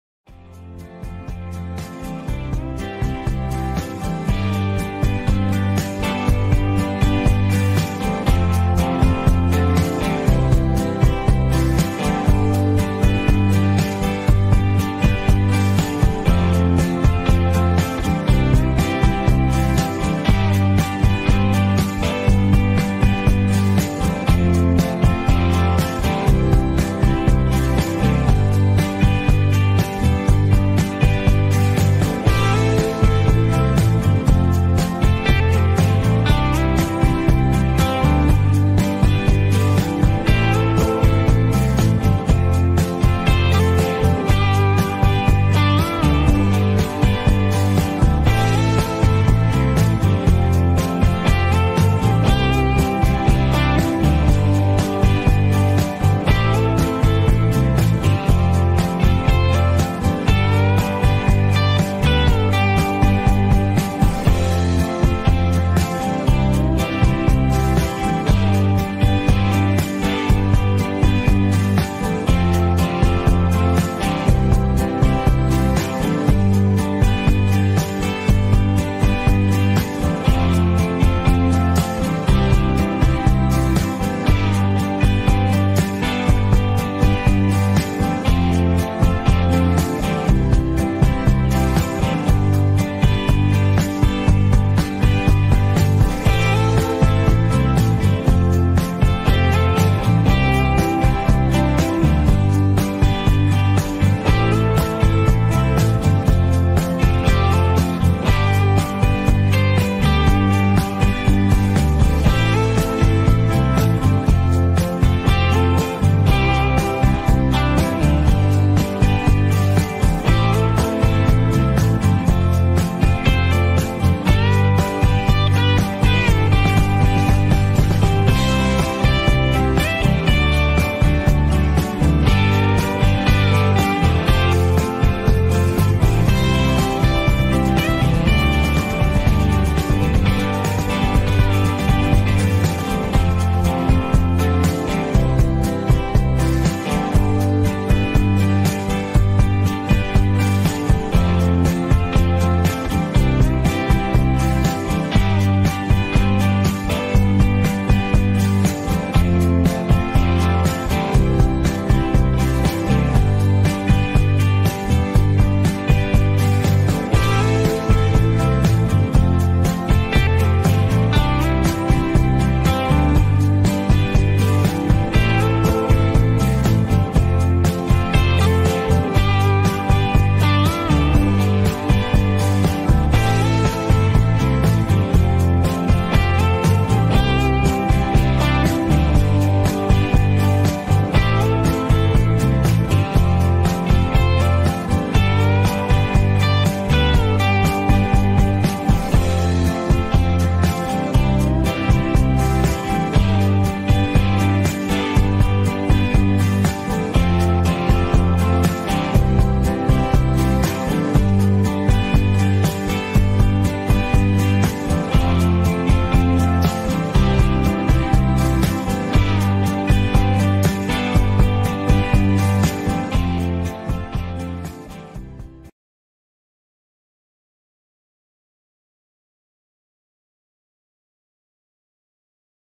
3 bedroom house for sale in Langebaan Country Estate | Pam Golding Properties
A modern kitchen strikes one upon entering the home and flows seamlessly onto multiple living areas with stacker doors that open up to a wooden deck that takes advantage of the beautiful golf course vistas.